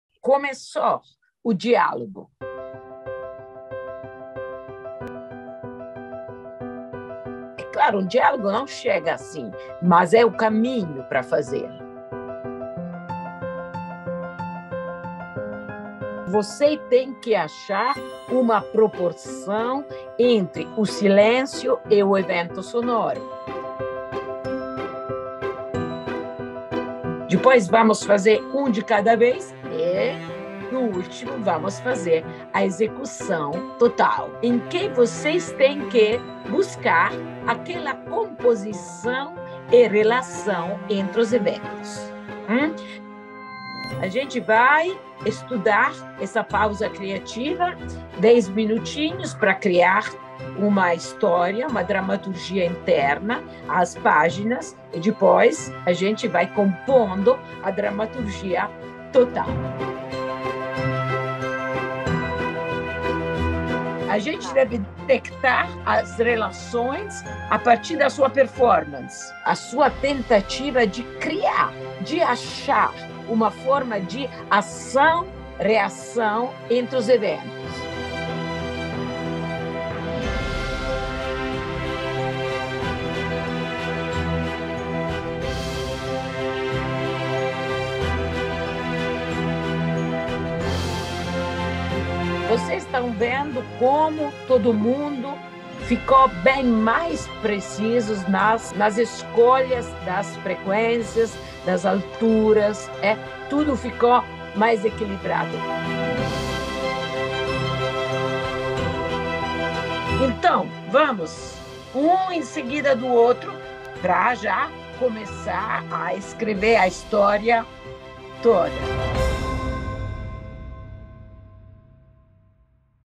CURSO DE VOZ ONLINE PARA ATORES COM BABAYA E FRANCESCA DELLA MONICA | PRIMEIRO SEMESTRE DE 2022
Agora com modalidade online, as mestras da voz Babaya e Francesca Della Monica estarão realizando aulas de voz, abordando suas respectivas metodologias de trabalho. As artistas conduzirão trabalho vocal com grupo exclusivo de dez atores, com foco no estudo de texto e de canções, além de abordar acerca dos fundamentos de suas pesquisas. O curso acontecerá de 18 de março à 27 de maio de 2022, sempre nas sextas-feiras, de 10h às 12h (Horário de Brasília). Serão dez encontros conduzidos por duas grandes pesquisadoras da voz do Brasil e da Itália, em uma experiência consistente com carga horária de vinte horas.